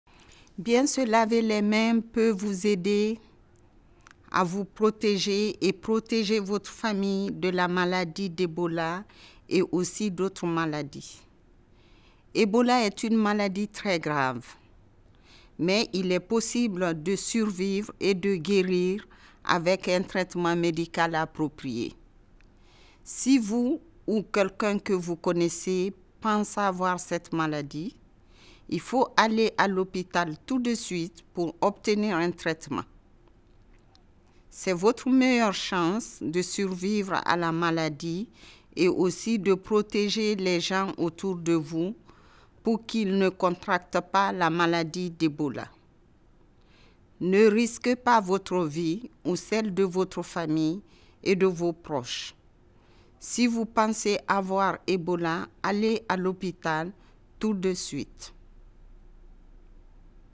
Ebola PSA from First Lady of Guinea, Hadja Djene Kaba Condé (in French)
(In French) The First Lady of Guinea, Hadja Djene Kaba Condé, delivers a PSA about Ebola. Washing hands well can help protect yourself and your family from Ebola and also other diseases. Ebola is a very serious disease. But it is possible to survive and heal with proper medical treatment. If you or someone you know thinks he has the disease, go to the hospital immediately for treatment.